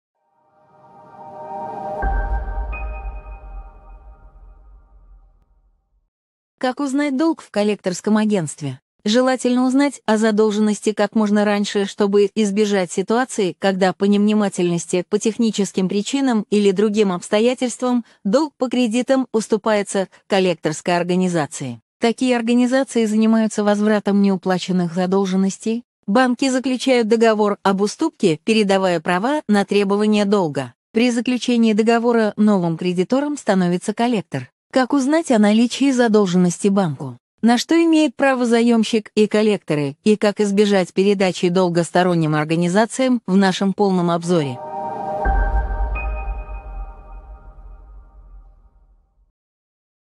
Как узнать в каком коллекторском агентстве долг
Как узнать о наличии задолженности в банке, на что имеют право коллекторы и как избежать передачи долга сторонним организациям, подробнее